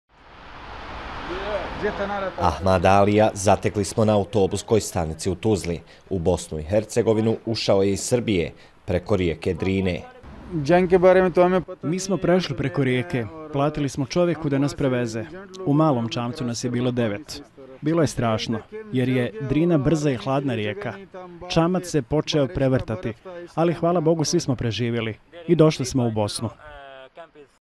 Migrant iz Afganistana opisuje prelazak bh. granice: Drina je bila brza, čamac se počeo prevrtati...
Ahmada Aliha zatekli smo na autobuskoj stanici u Tuzli. U Bosnu i Hercegovinu ušao je iz Srbije, preko rijeke Drine.